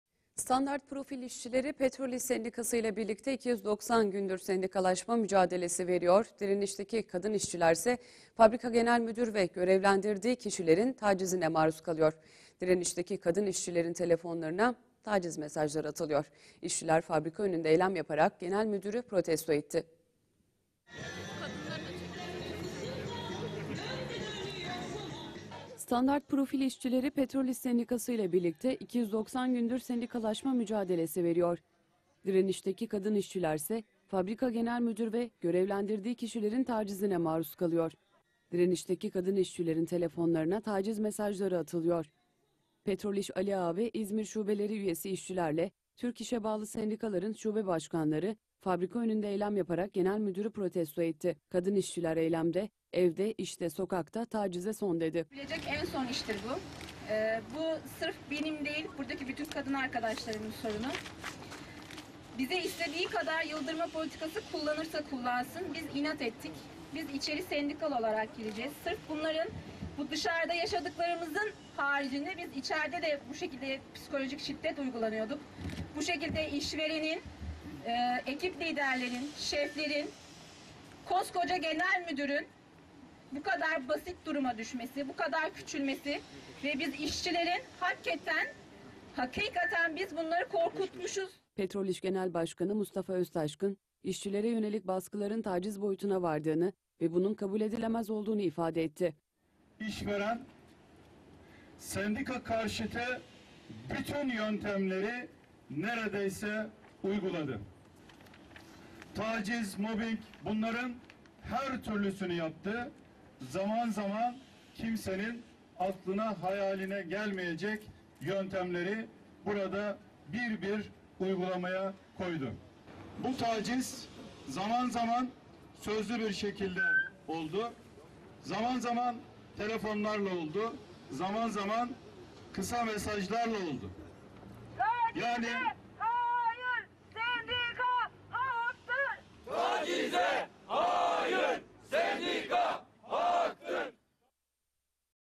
Standart Profil Genel Müdürüne taciz protestosu
Petrol-İş Sendikası, 290 gündür sendikalaşma mücadelesi veren Standart Profil işçileri ile birlikte fabrika önünde yaptığı açıklamayla, sendika üyesi kadın işçilere taciz mesajları gönderen fabrika Genel Müdürü Sebahattin Karabulut'u protesto etti. Eyleme Petrol-İş Aliağa ve İzmir Şubelerine üye 150 dolayında işçi ile Türk-İş'e bağlı sendikaların İzmir Şube Başkanları, Manisa'dan işçi ve emekçiler ile sendikacılardan oluşan 200 dolayında kişi katıldı.